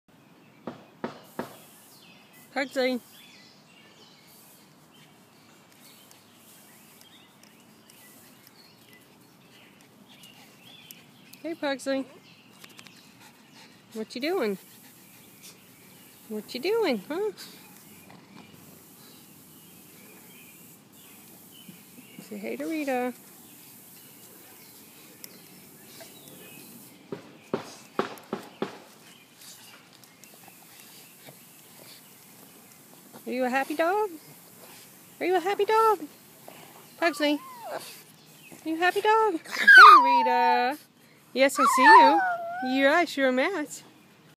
Happy Dog the Puggle Gets Photo Bombed
Here is a kinda funny video of Happy Dog (aka Pugsley) getting photo bombed by Rita the talking pug.